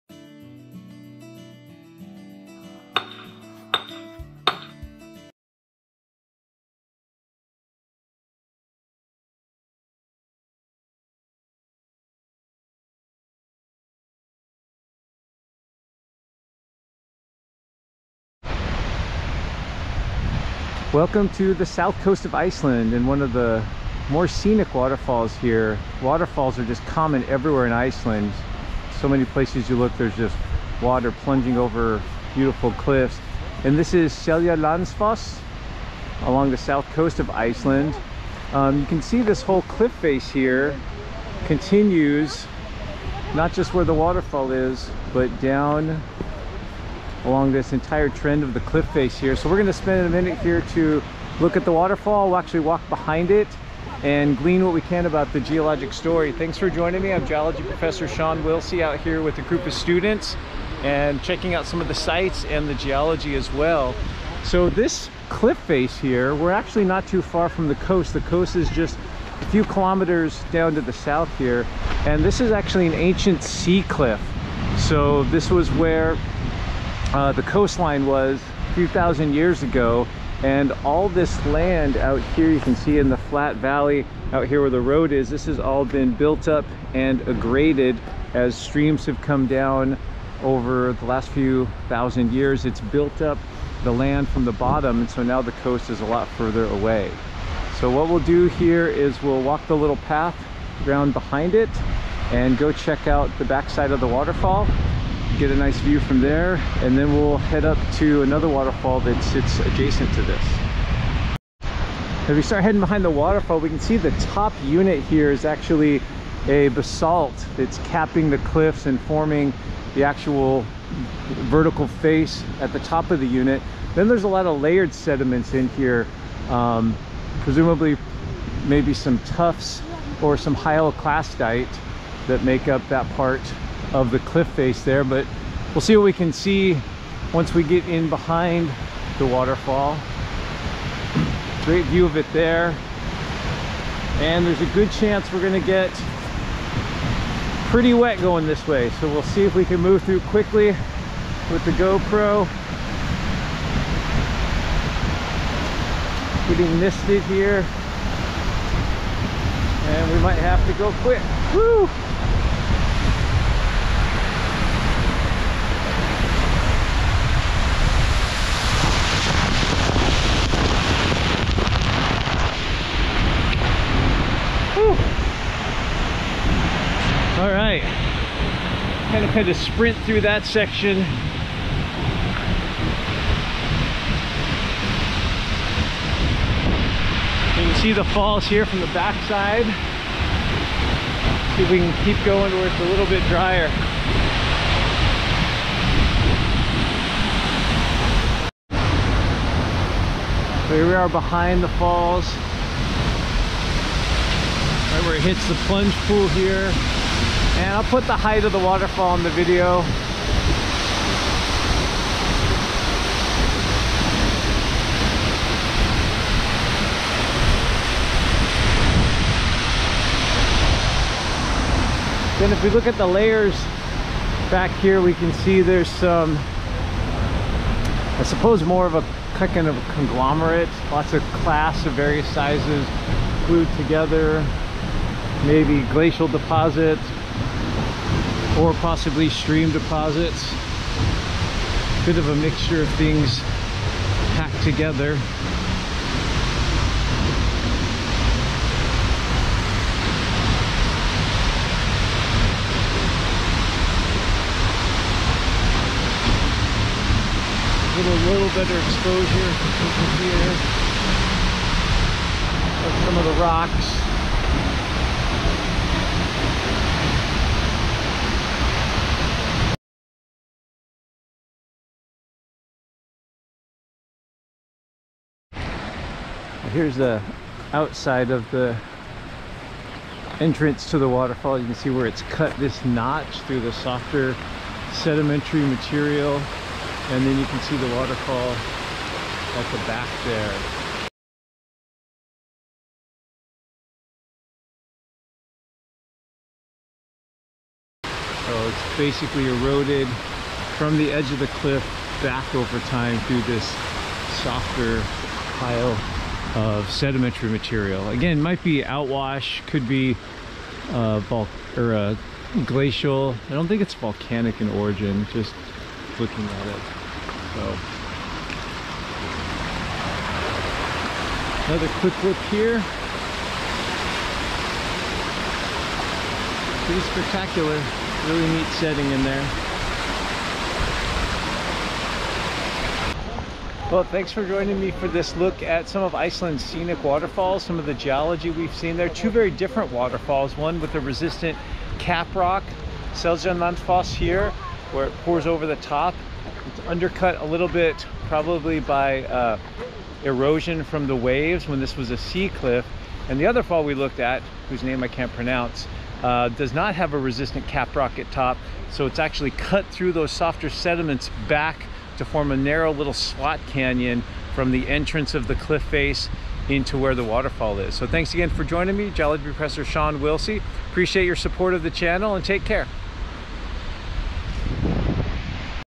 Comparing Two of Iceland's Iconic Waterfalls: Seljalandsfoss and Gljúfrabúi
Head to the south coast of Iceland with geology professor Shawn Willsey to investigate two adjacent but different waterfalls, Seljalandsfoss and Gljúfrabúi.

or click on the "Thanks" button above
or a good ol' fashioned check to:
Shawn Willsey
148 Blue Lakes Blvd N